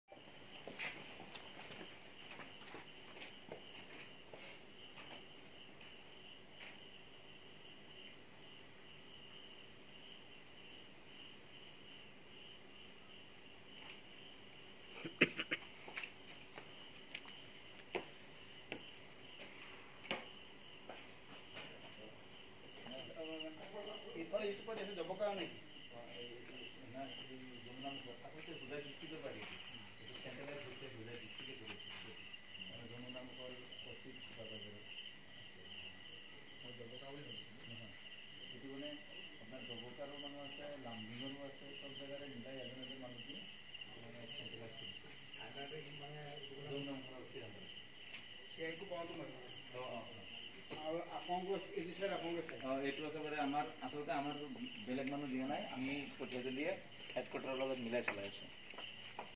কঠিয়াতলী নেপালী খুটিত বৃহৎ পৰিমাণৰ কাঠ জব্দ
KATHIYATOLI NEPALIKHUTI WOOD SEIZED